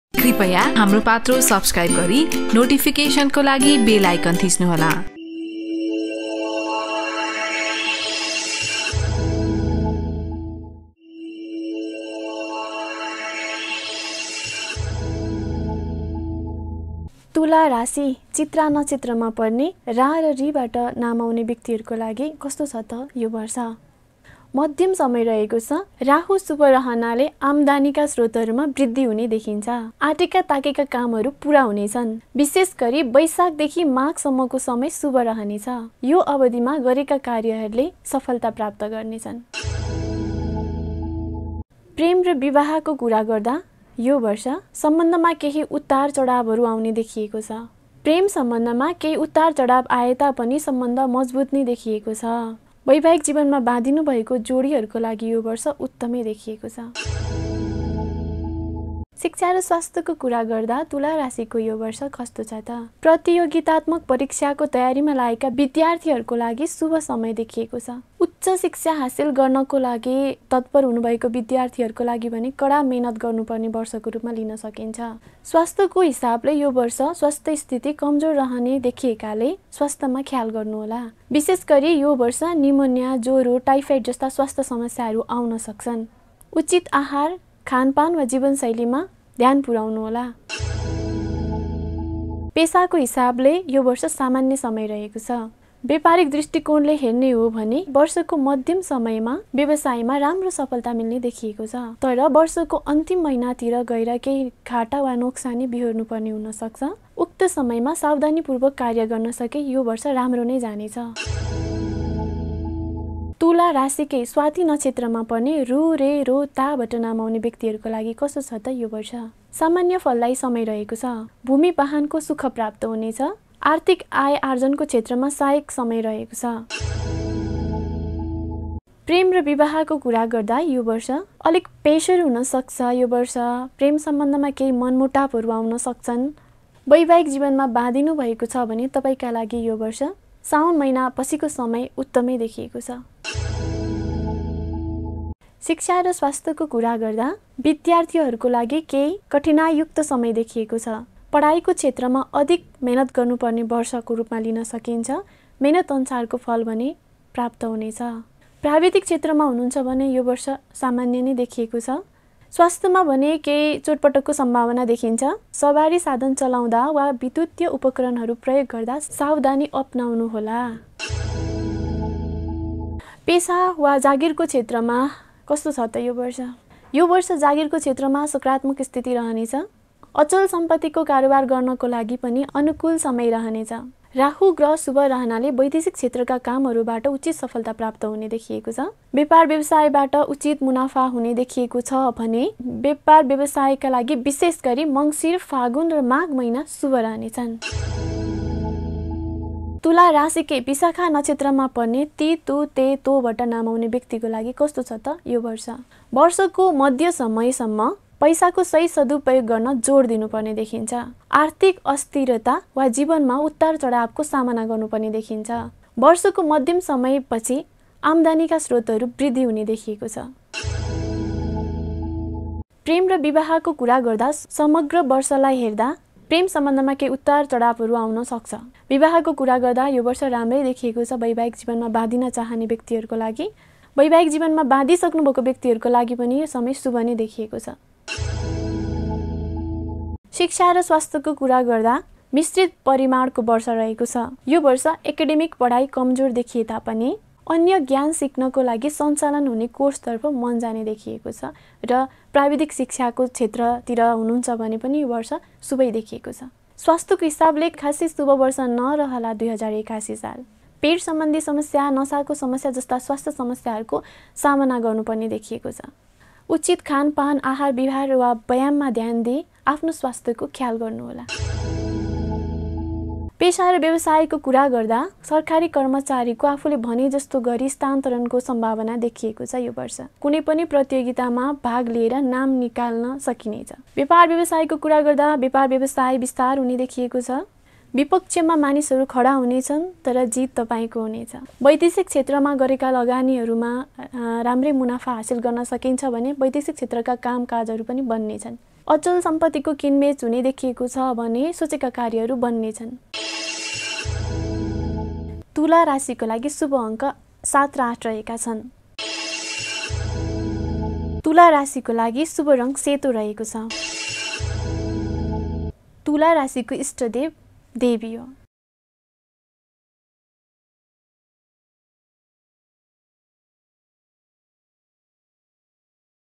Tula 2081 Nepali Rashifal | वार्षिक राशिफल २०८१ - तुला । हाम्रो पात्रो | HamroPatro | HamroRashifal
तुला राशी भएका मानिसहरुको लागि २०८१ साल कस्तो रहनेछ त ? व्यापार व्यवसाय, प्रेम सम्बन्ध, शिक्षा, पेशा/ रोजगार लगायतका क्षेत्रमा २०८१ साल कतिको लाभदायी रहनेछ भनेर जान्नुहोस् ।

Get ready for the year ahead with our comprehensive horoscope for 2081 BS. Our astrology experts have analyzed the zodiac signs to provide accurate forecasts and astrological predictions. Discover what the stars have in store for you and gain insight into your future with our in-depth horoscope.

Know how Tula (Libra) horoscope will work out in 2081 B.S.